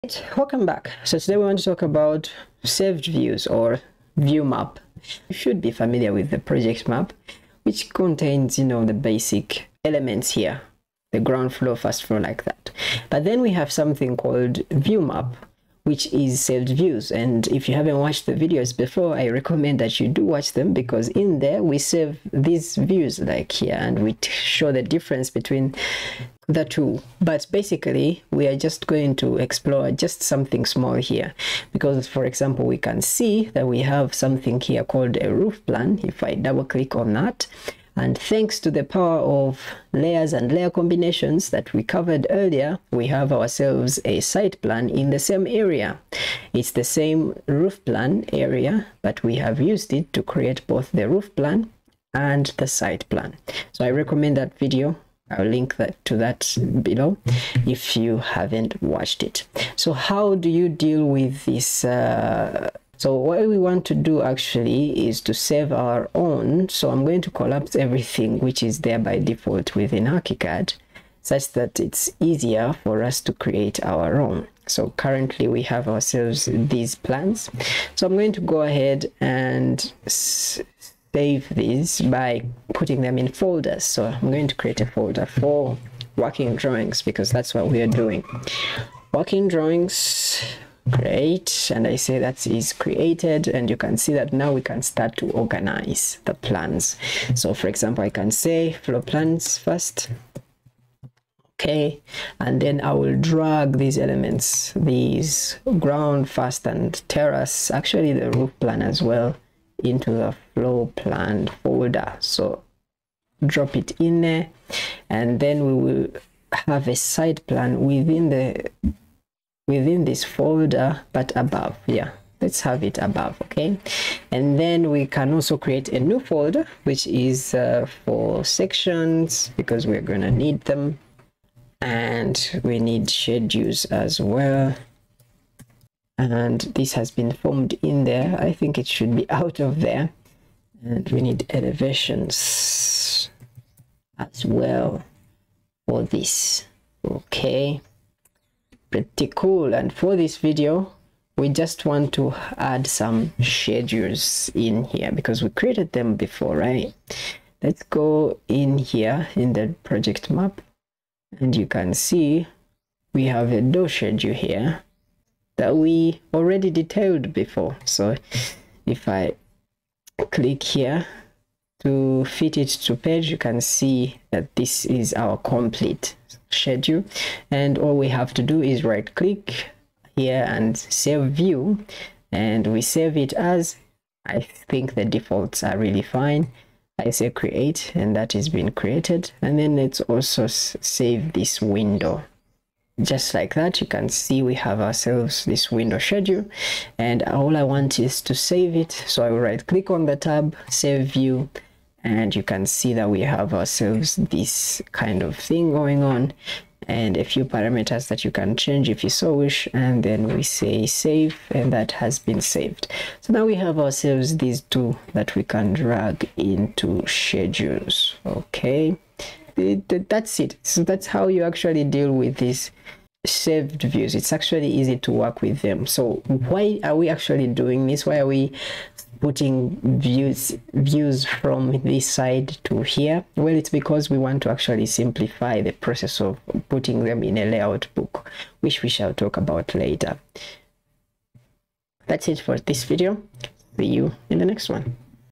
View Map | 015 Working drawings | Archicad
In this video, we explore how to organize your drawings in the View Map